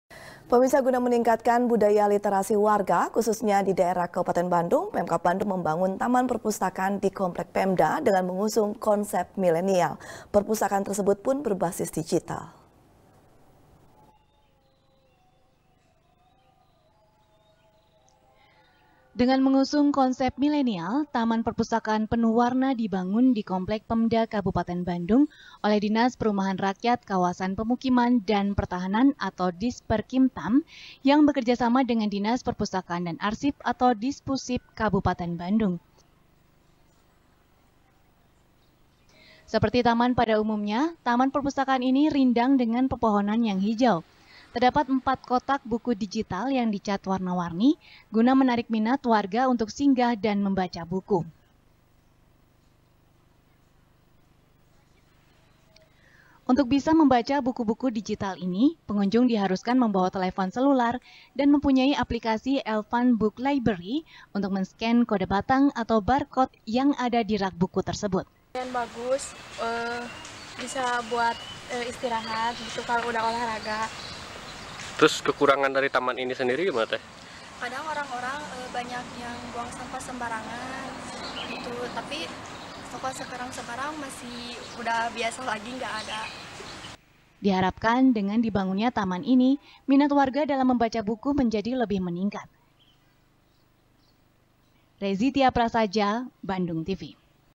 Tingkatkan Literasi, PEMKAB Bangun Taman Perpustakaan Digital | SBR | BANDUNGTV
SBR | BANDUNGTV | FARHAT MUMTAZ | Guna meningkatkan budaya literasi warga, khususnya di kabupaten bandung, pemkab bandung membangun taman perpustakaan di komplek pemda, dengan mengusung konsep milenial. Perpustakaan tersebut pun berbasis digital.